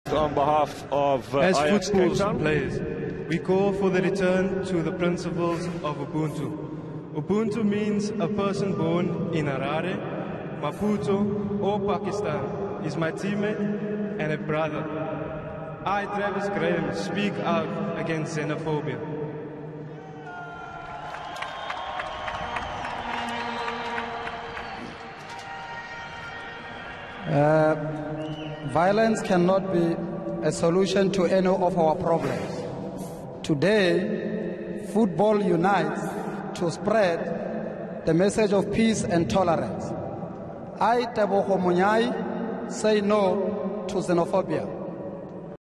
Wits vs Chippa United captains against Xenophobia
Travis Grahams & Tebogo Monyai